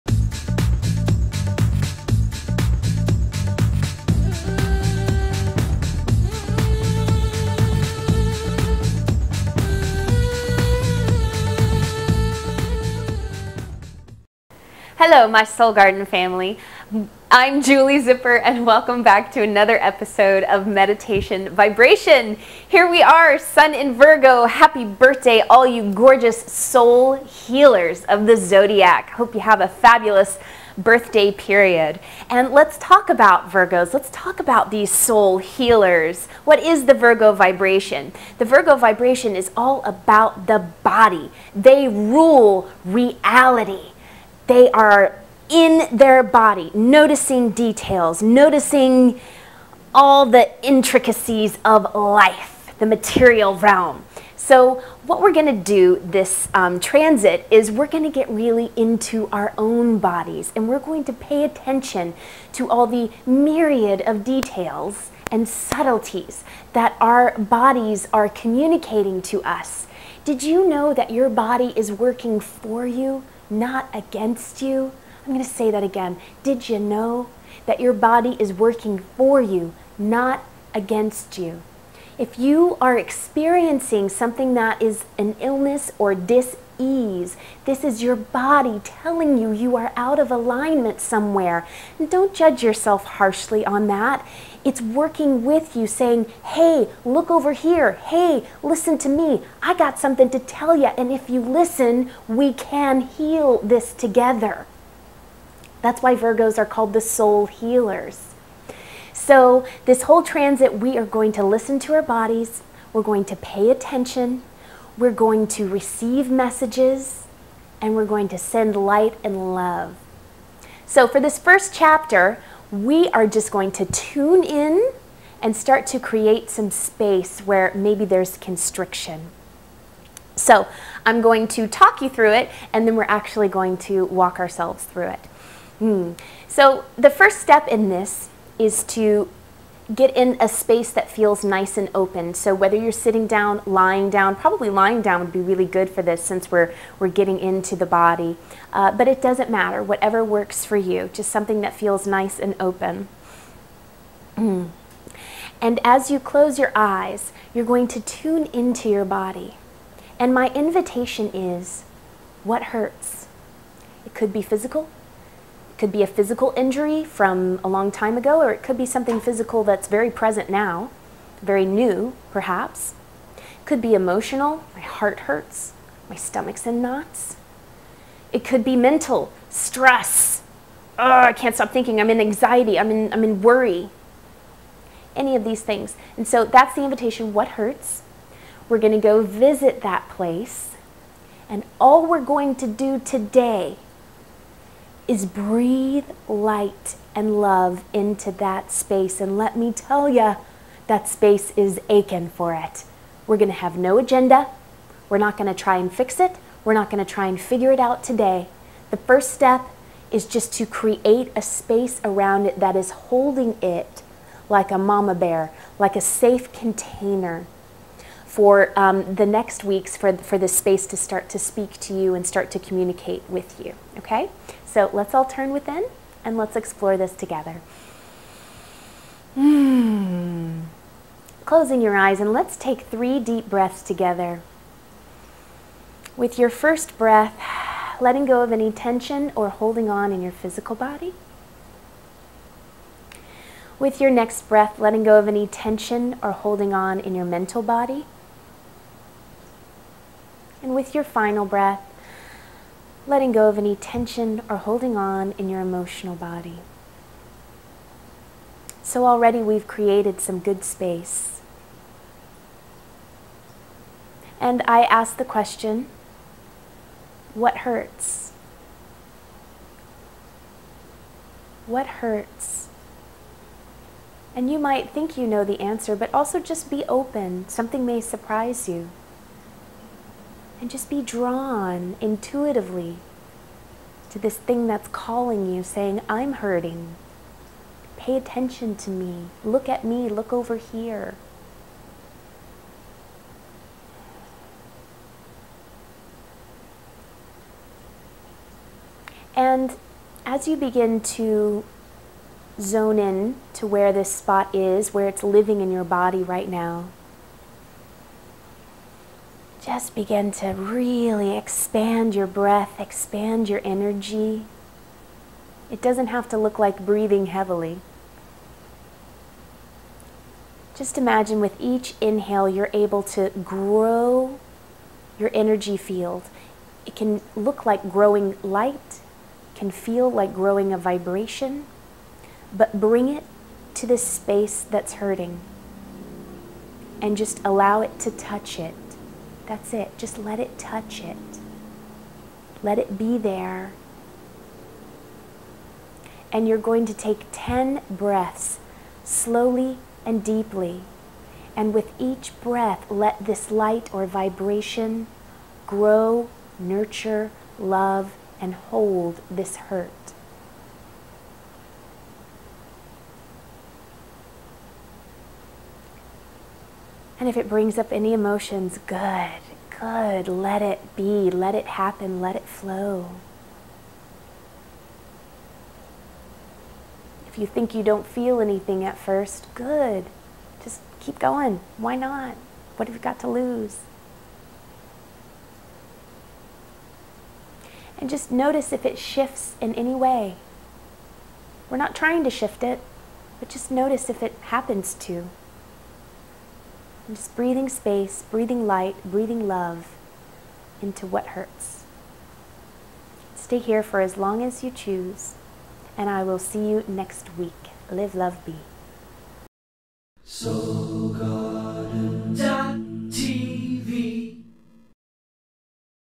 Meditation Vibration Virgo Chapter 1 w/Julie Zipper
Spiritual Counselor, Julie Zipper presents gentle guidance orbiting this month's astrology and provides a simple guided meditation to glide through the turbulence in your life.